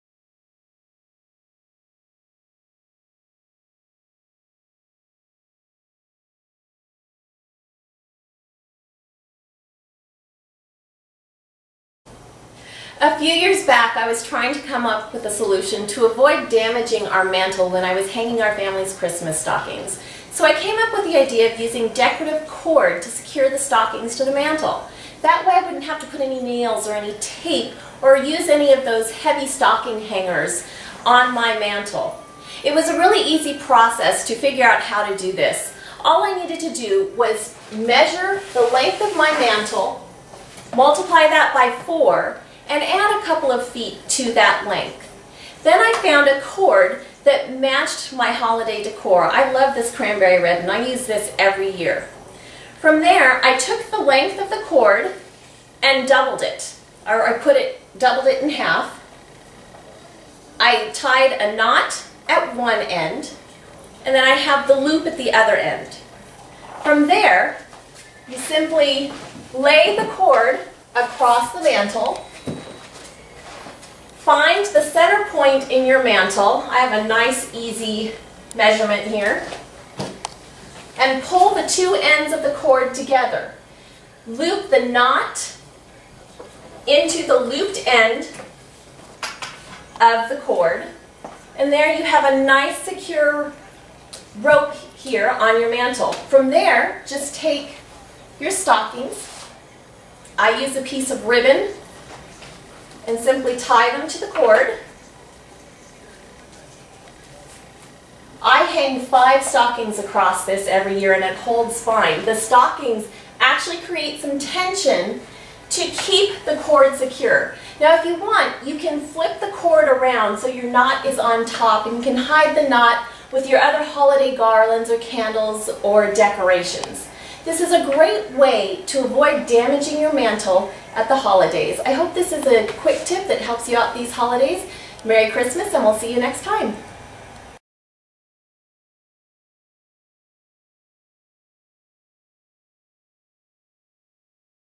DIY Diva Holiday Quick Tips: Save your Mantle!
Holiday tips for hanging Christmas Stockings without damaging your mantle.